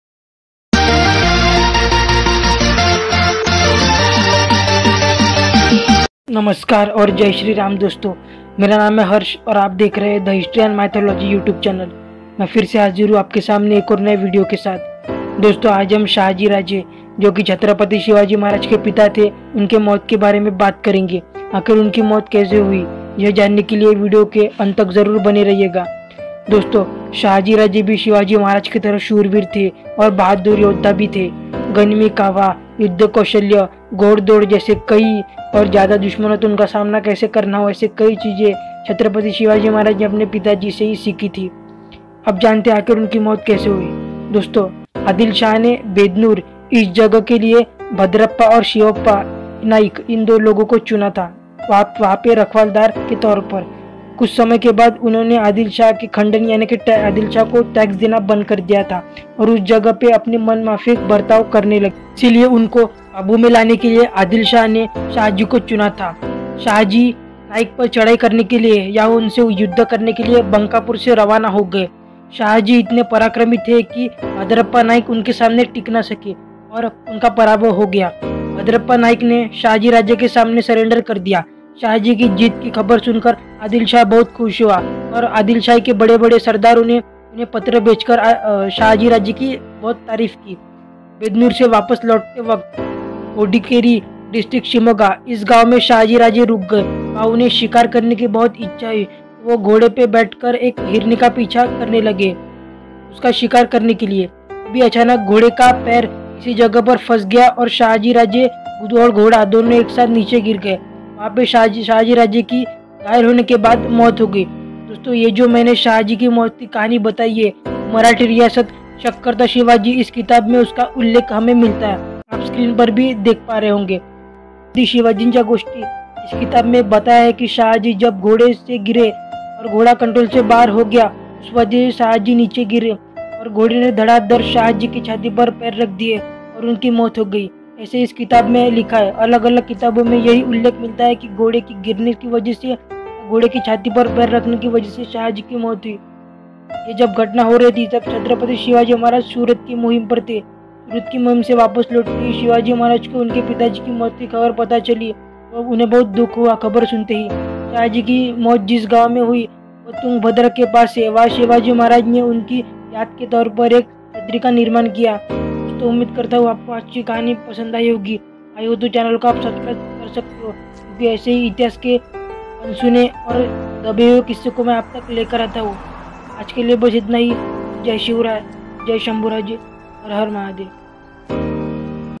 छत्रपति शिवाजी महाराज के पिता शहाजी की मौत कैसे हुई ? How shahajiraje was died?
छत्रपति शिवाजी महाराज के पिता शहाजी की मौत कैसे हुई ?
How shahajiraje was died?

source- marathi riyasat(शकर्कता शिवाजी) and chatrapati shivaji chya gosti, etc